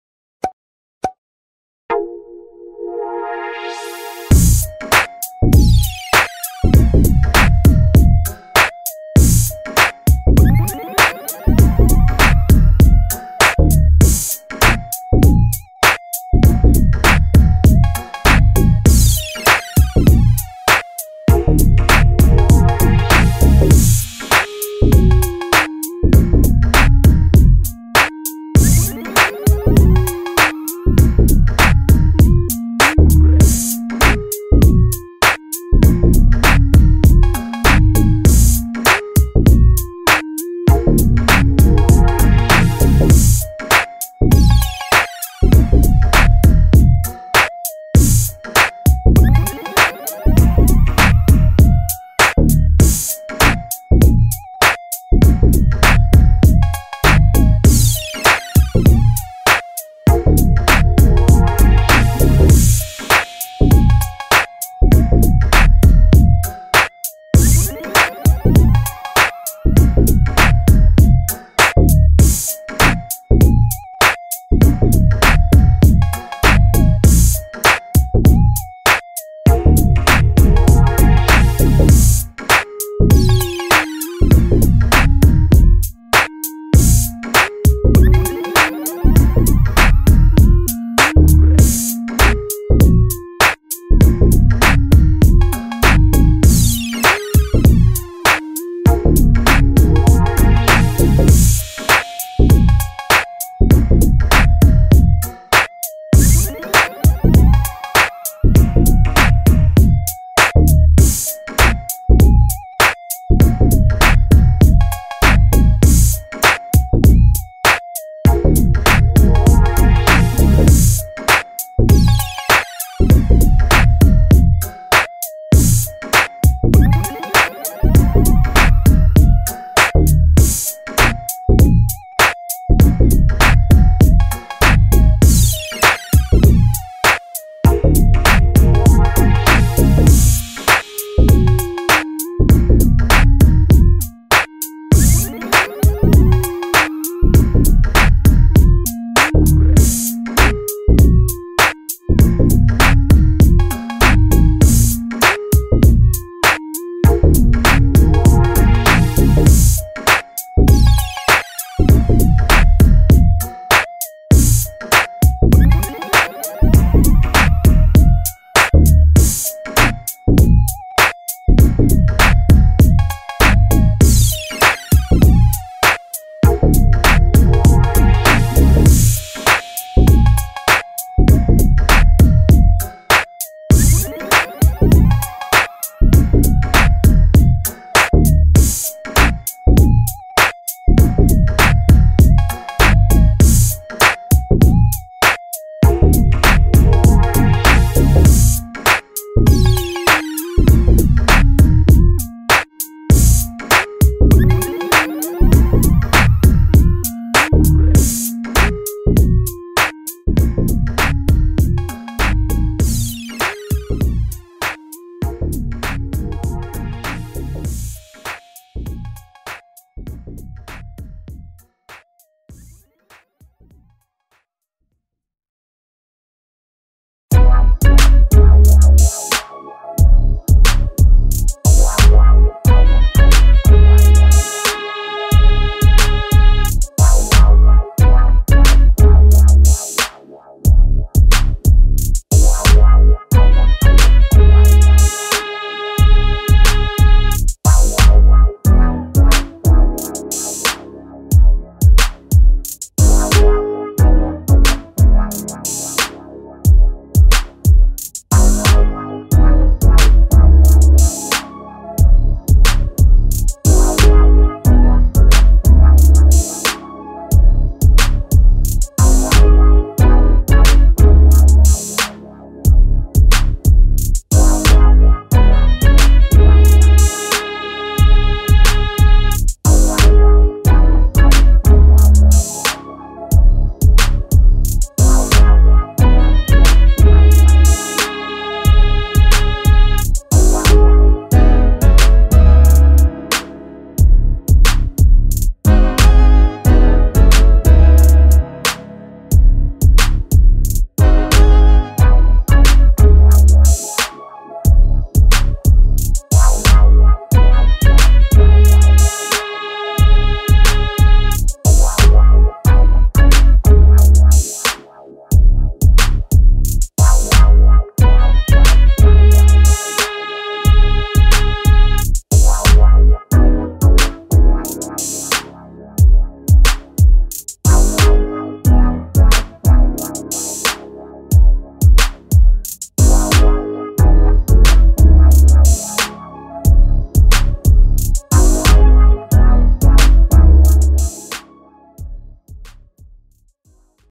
How to Draw STEVE MINECRAFT - Very Easy
In this video you will learn how to draw STEVE MINECRAFT